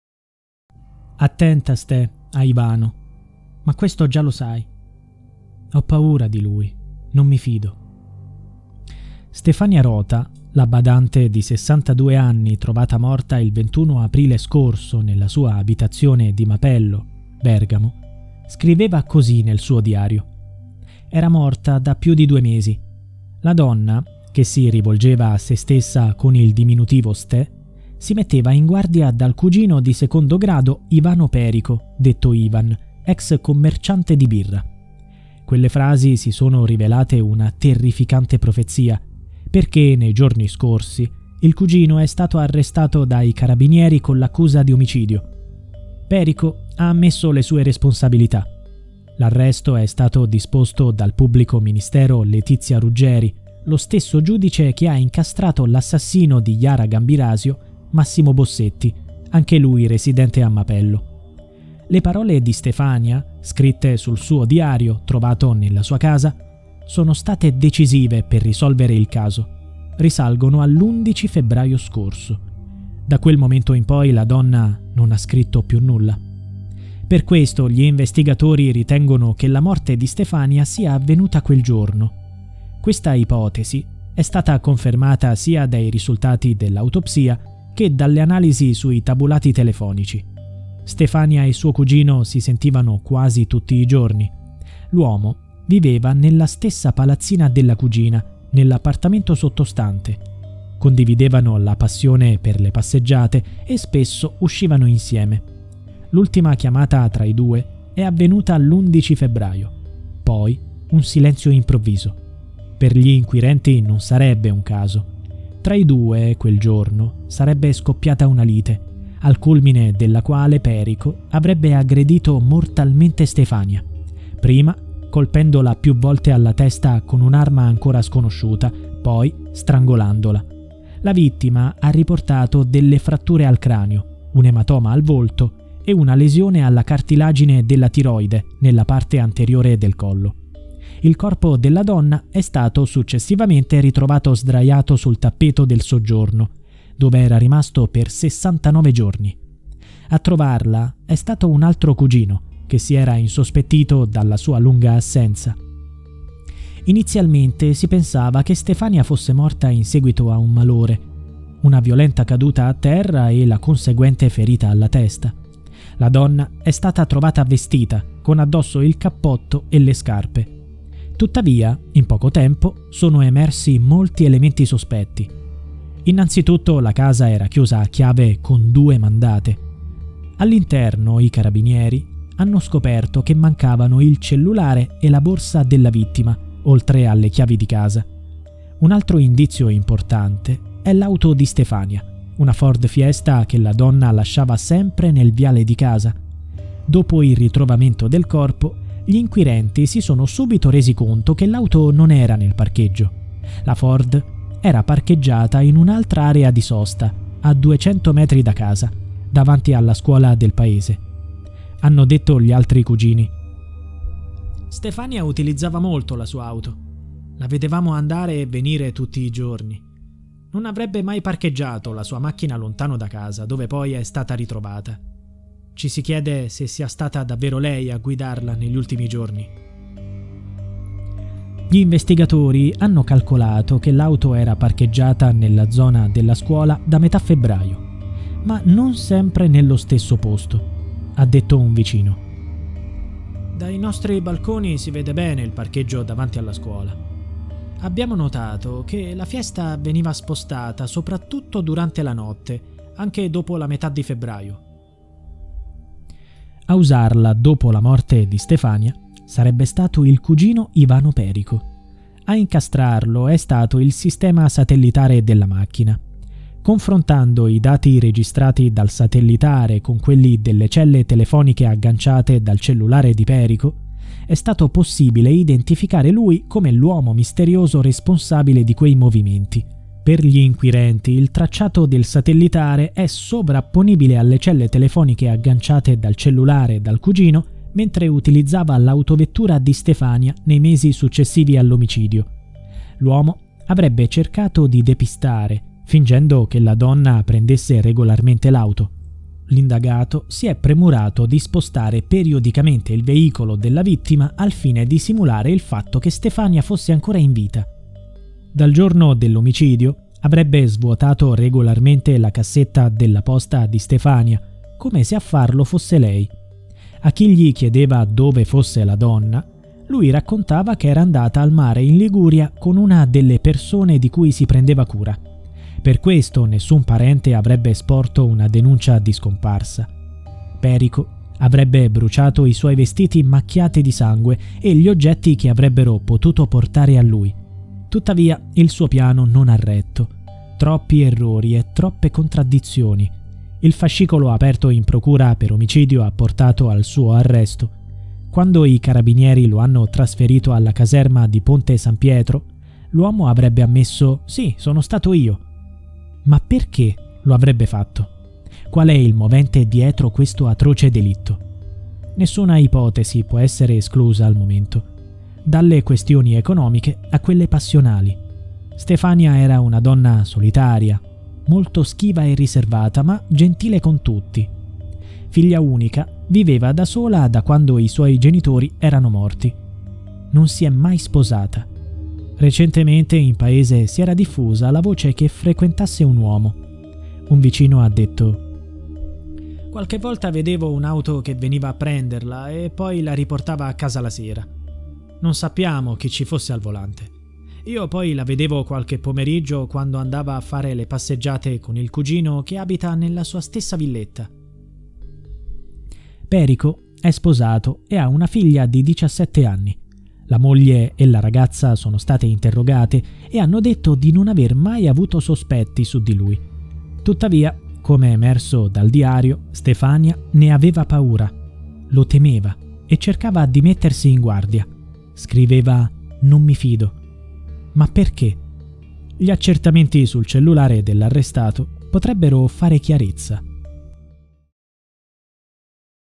Il cugino ha depistato le indagini - Stefania Rota
Clamorosa svolta nel giallo della badante trovata morta in casa da due mesi